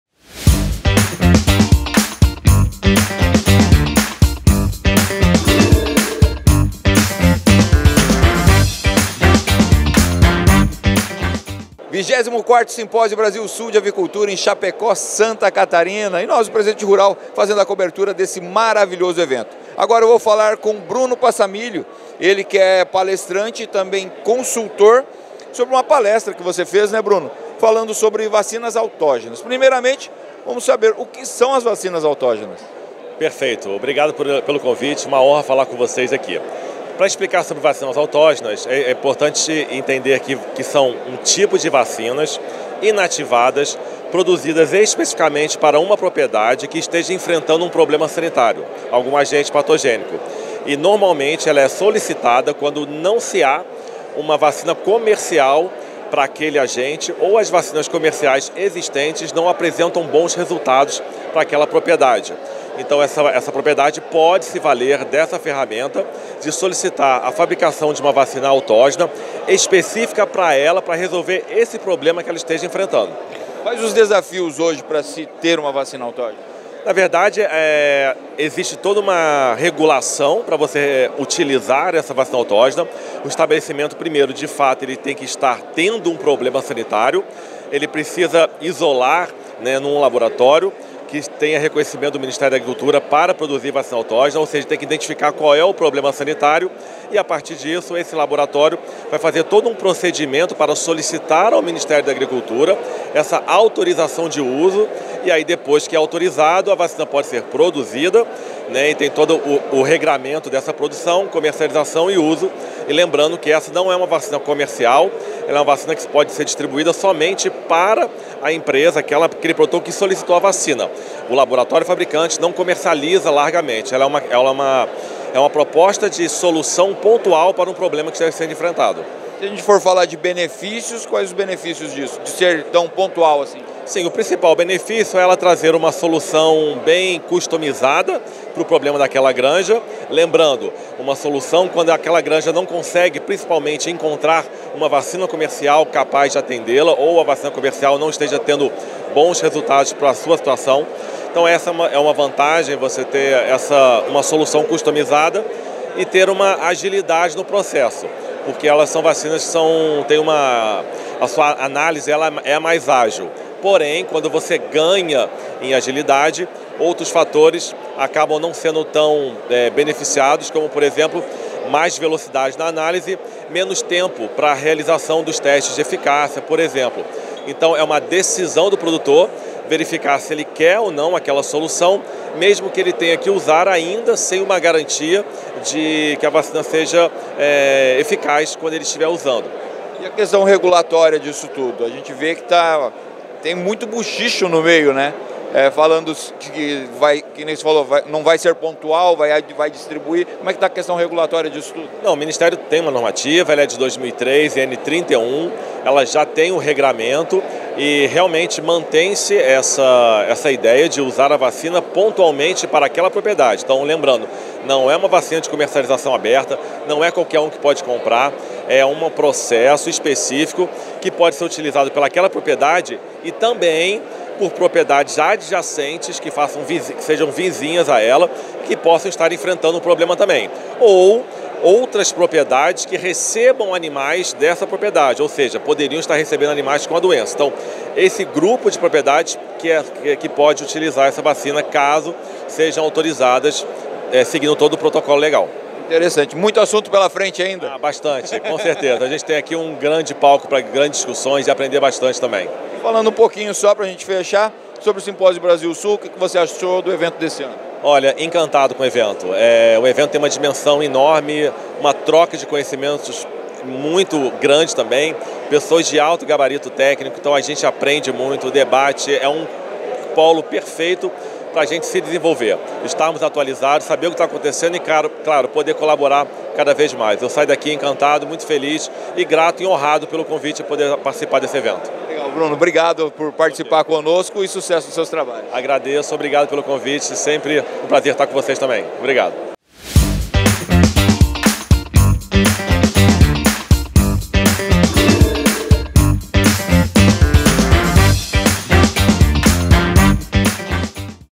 Saiba tudo sobre vacinas autógenas e suas regras
As vacinas autógenas têm ganhado cada vez mais espaço no mercado avícola brasileiro, mas é importante observar as regras. Produzidas especificamente para uma única propriedade, normalmente solicitada se as vacinas comerciais não geram resultados satisfatórios, elas oferecem uma solução customizada. Quem fala sobre o assunto Bruno Pessamilio, palestrante do 24º Simpósio Brasil Sul de Avicultura, de 09 a 11 de abril, em Chapecó, SC.